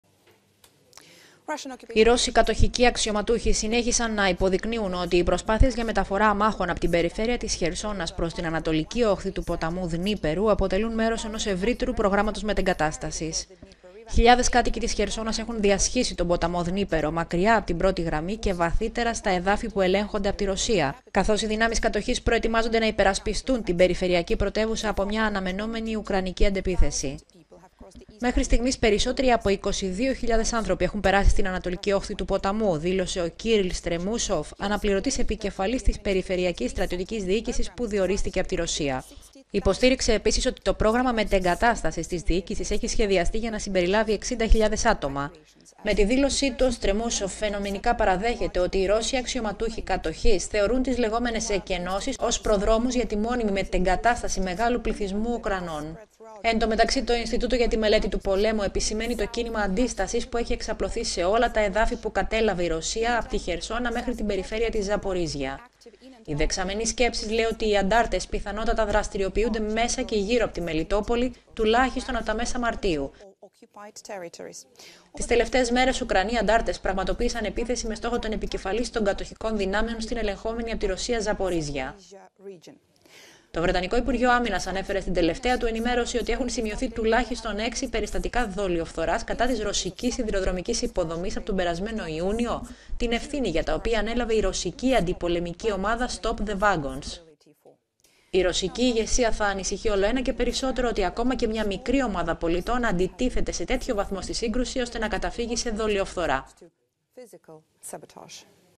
Πόλεμος στην Ουκρανία: Η κατάσταση στο έδαφος (χάρτες)
Χιλιάδες κάτοικοι της Χερσώνας έχουν διασχίσει τον ποταμό Δνείπερο, μακριά από την πρώτη γραμμή και βαθύτερα στα εδάφη που ελέγχονται από τη Ρωσία